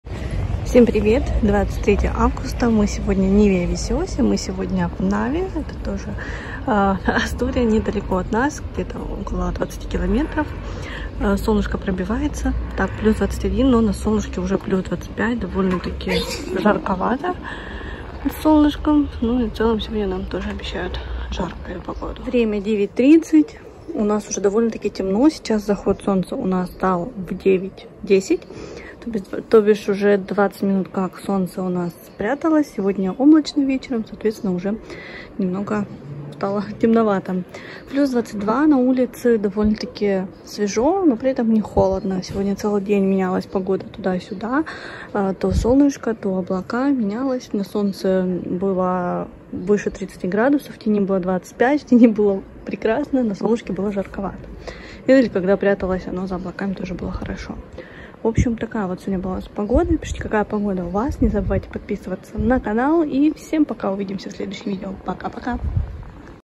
ПОГОДА В АСТУРИИ. 23 августа 2022 г. СЕВЕР ИСПАНИИ
Такая вот у нас погода по состоянию на 23 августа 2022 года